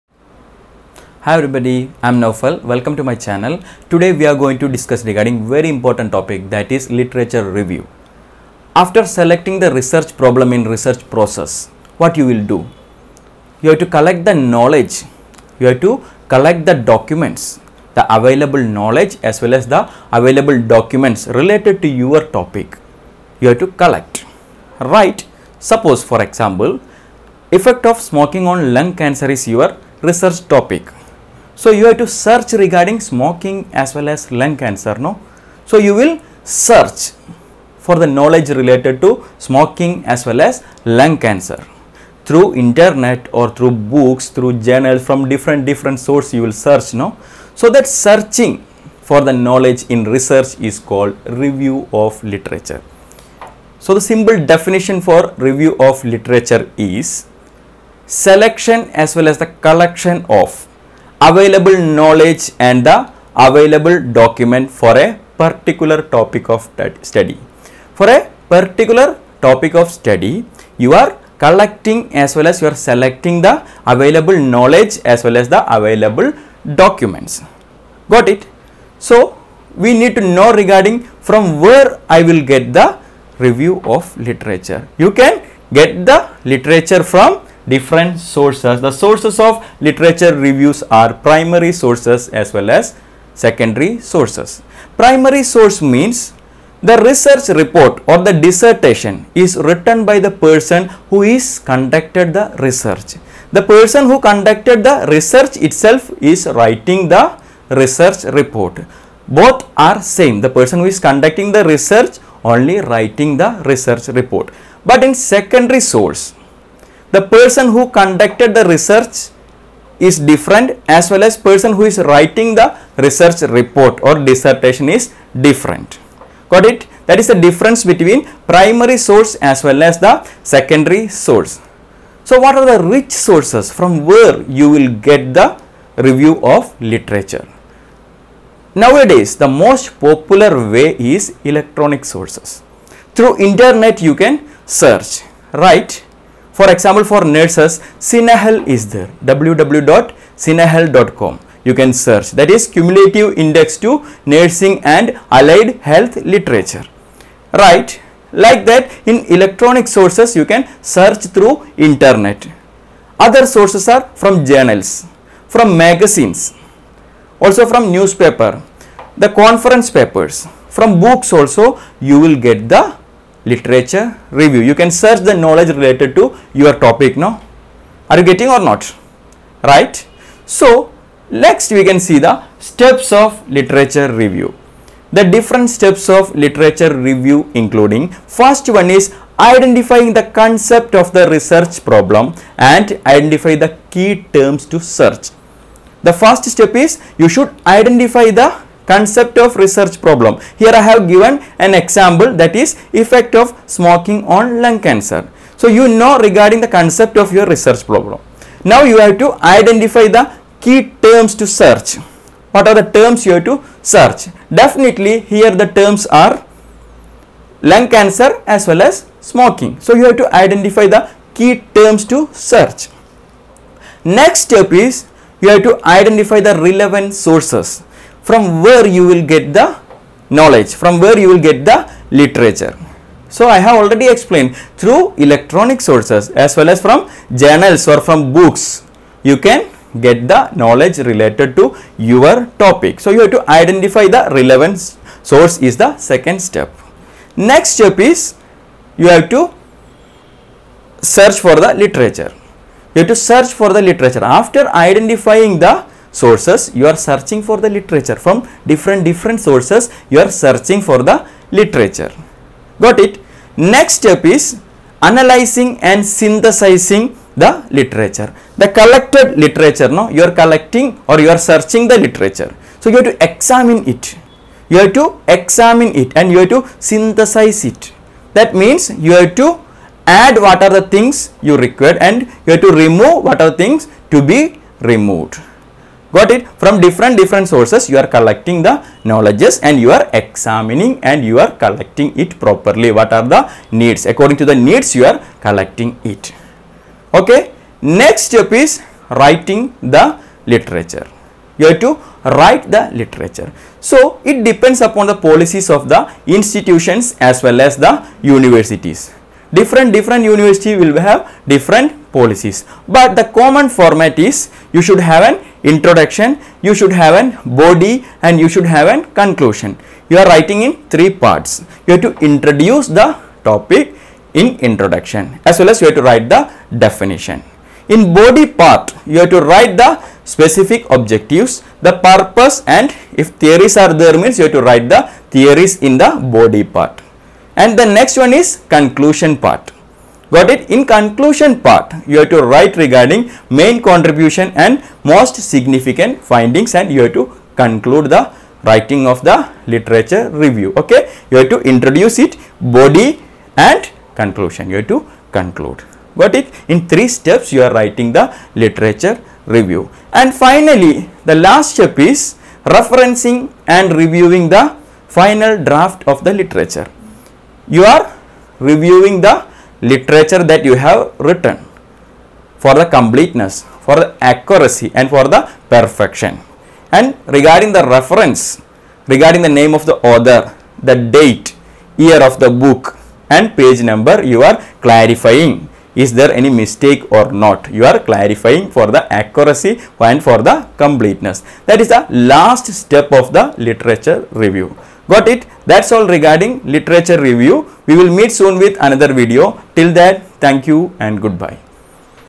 Steps and sources of literature review :simple and easy explanation
Simple explanation for review of literature, what is meant by literature review, different sources of literature review and steps of literature review and how to write literature review